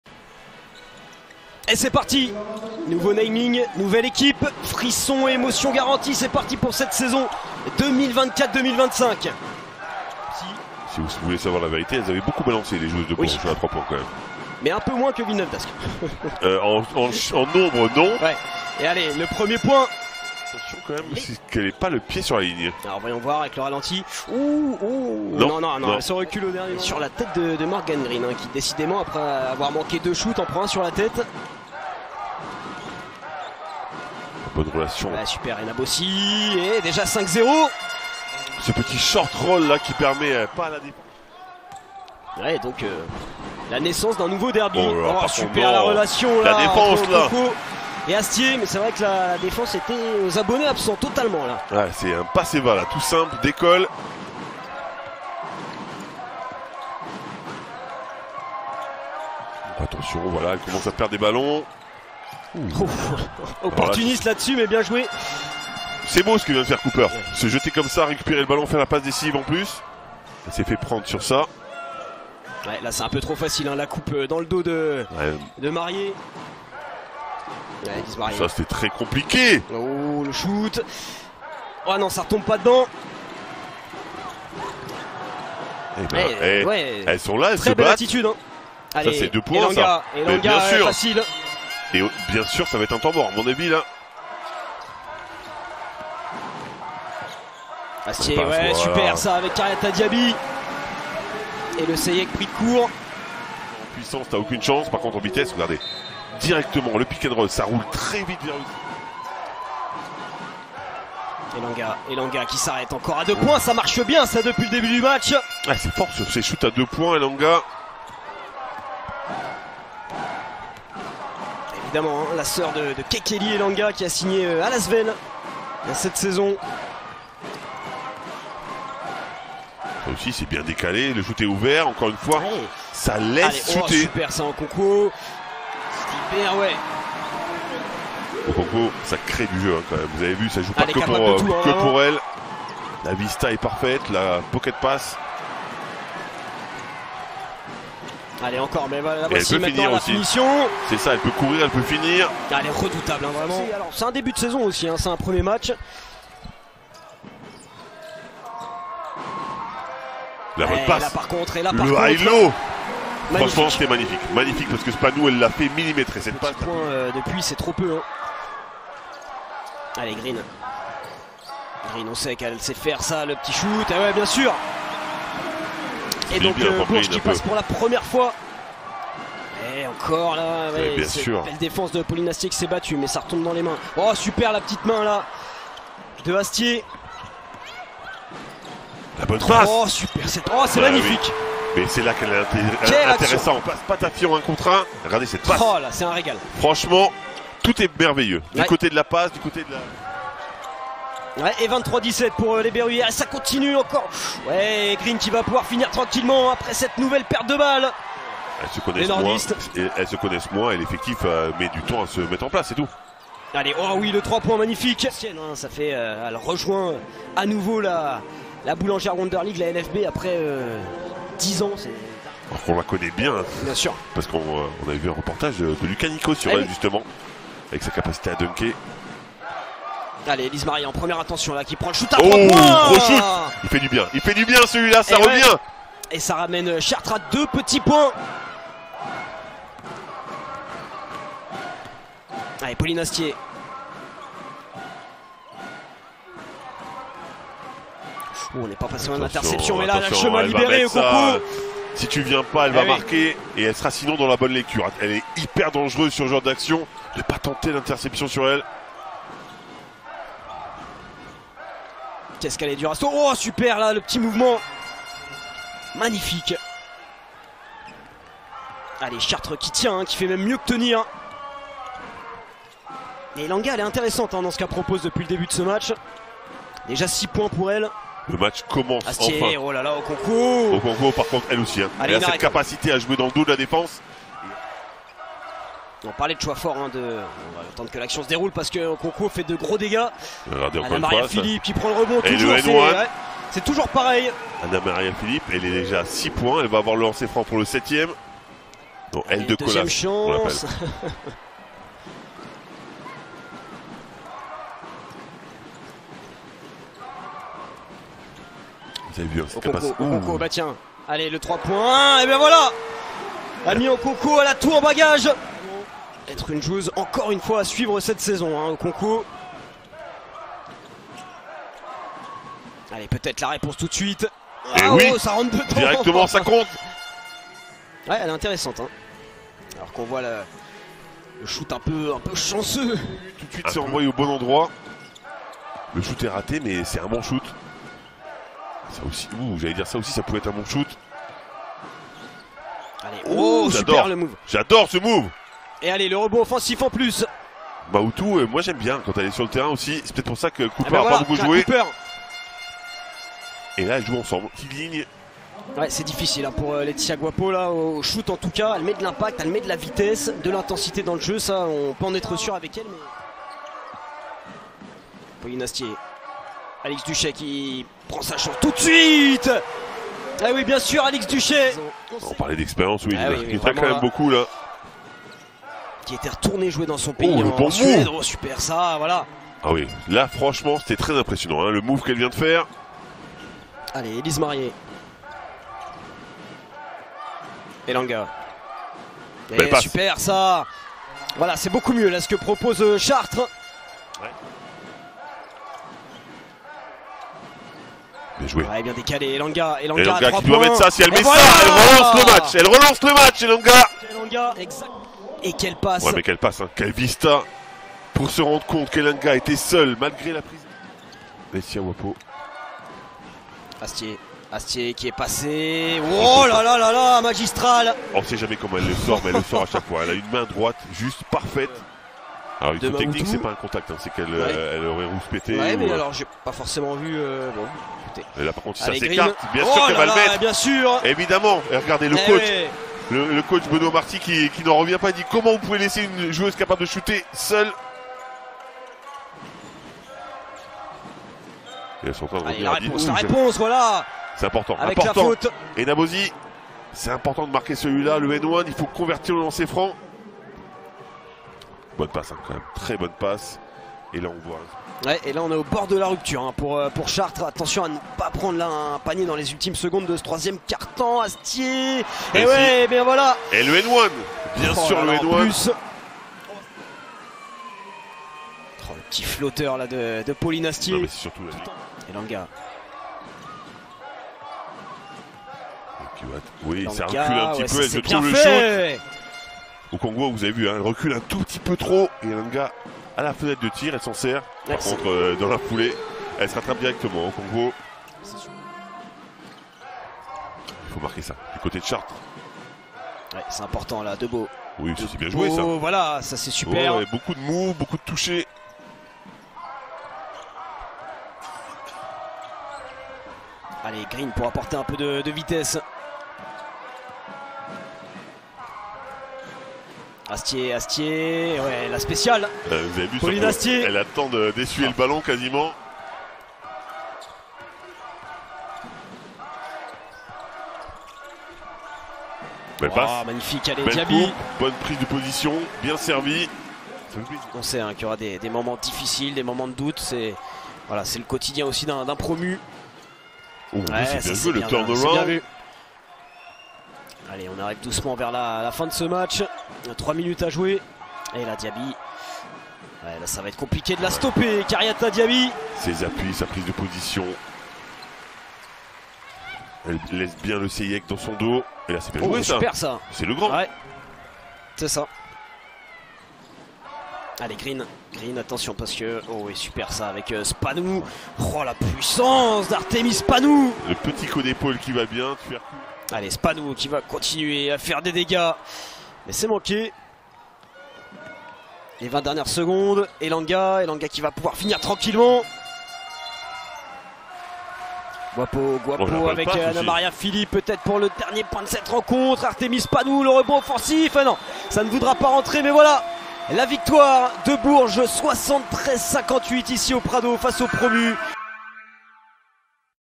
🏀 Le résumé de BOURGES - CHARTRES (J1) | La Boulangère Wonderligue 2024/25
Retrouvez le résumé du premier match de la saison de La Boulangère Wonderligue 2024-25 diffusé sur Sport en France 🏀